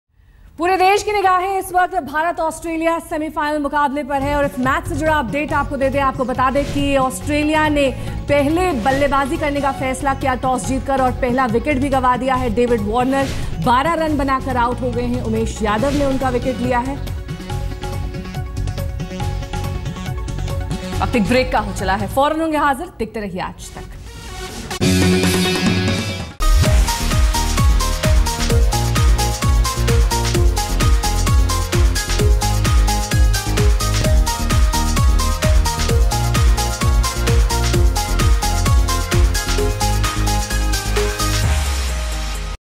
ICC World Cup 2015: David Warner Out At 12
Australia loses its first wicket, David Warner is out at 12 runs. Umesh Yadav got the wicket.